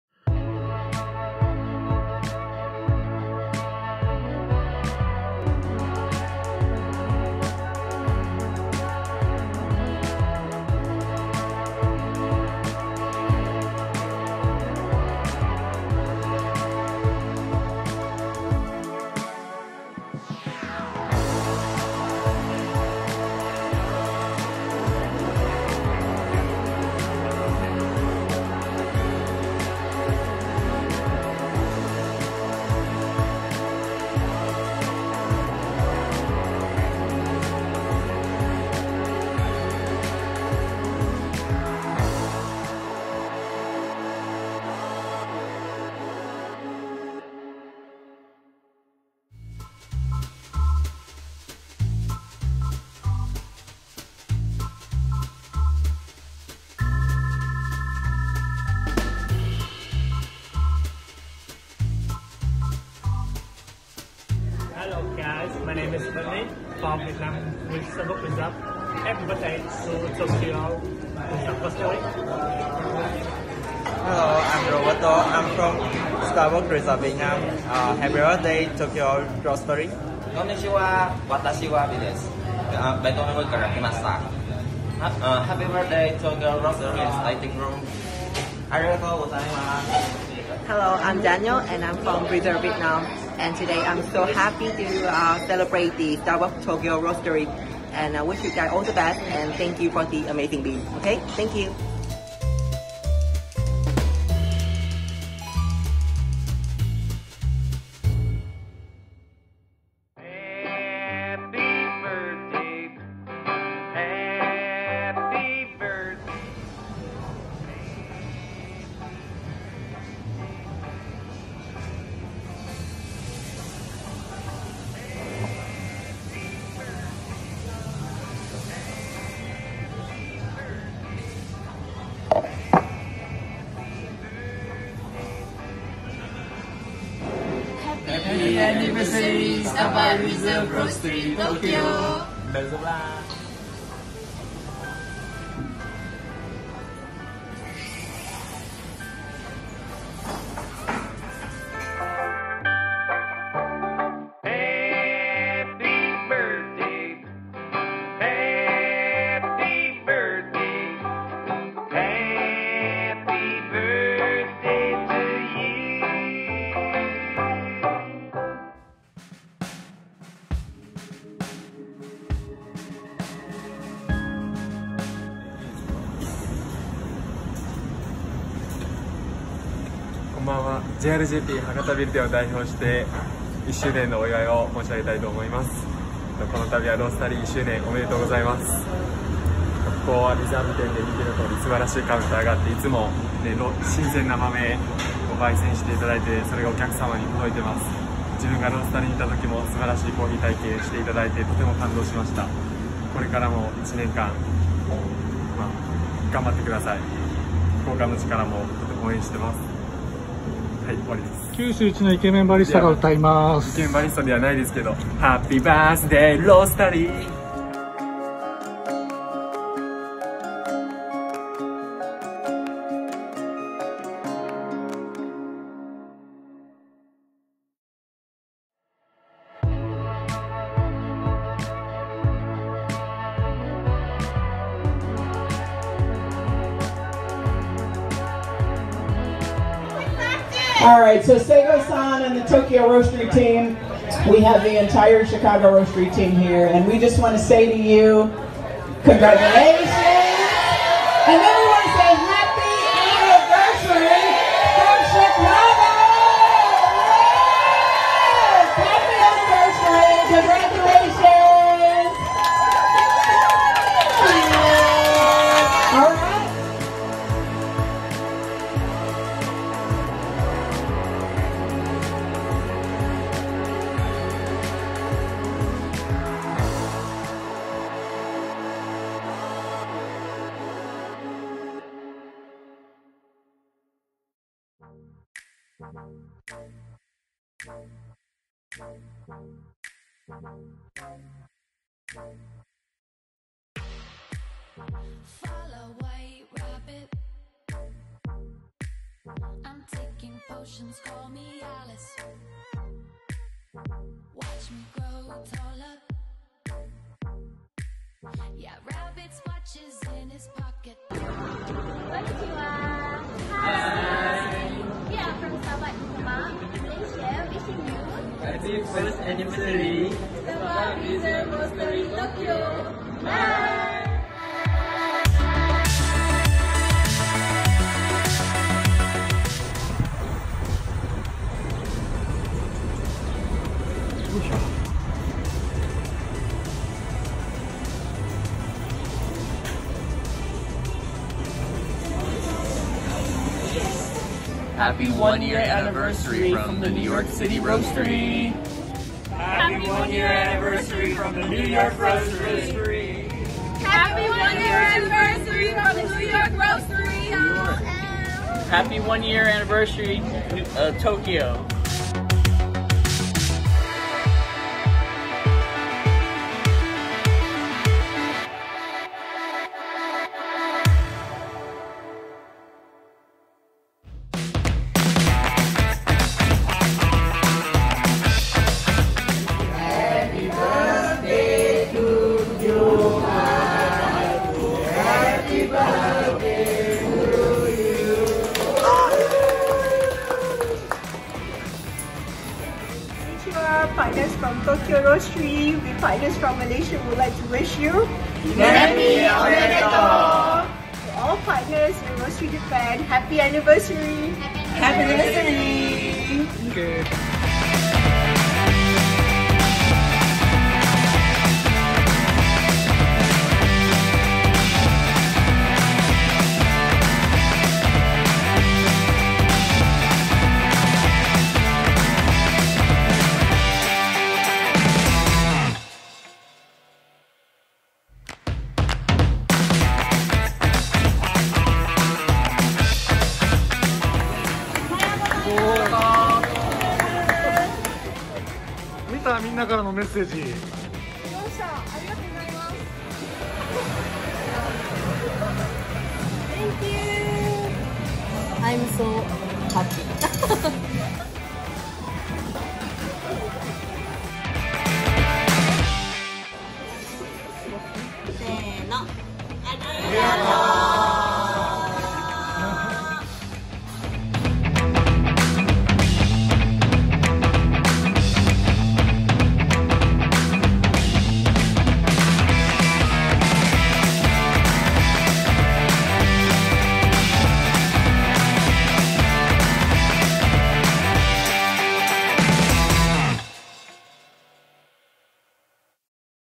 【1周年お祝いムービー】STARBUCKS RESERVE ROASTERY TOKYO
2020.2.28
スターバックスリザーブロースタリー東京が1周年を迎えました。
各国のパートナーからお祝いのムービーが届きました。
最後には東京からのお礼ムービーも。

国は違っても、スターバックスのパッションは世界共通です！

Instagramで私のスターバックス巡りを紹介しています。